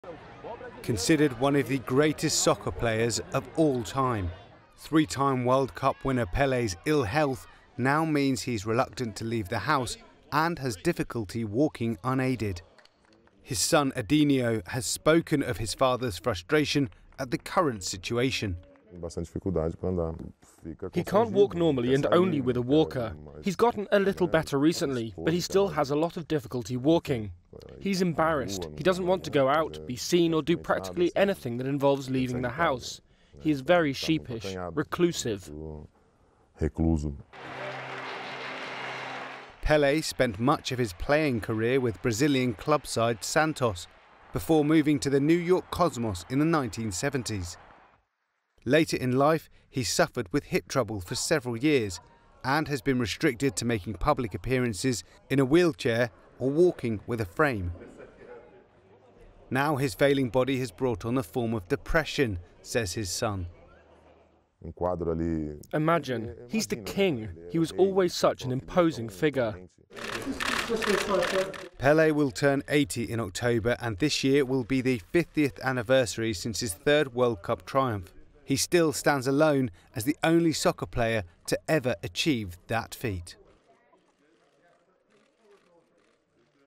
Ageing football icon Pele depressed, reclusive: Son | The Straits Times
Brazilian soccer great Pele is depressed over his poor health and reluctant to leave the house because he cannot walk unaided, his son has said in an new interview.

Considered one of the greatest soccer players of all time, three time World Cup winner Pele's ill health now means he's reluctant to leave the house and has difficulty walking unaided.

His son Edinho has spoken of his father's frustration at the current situation.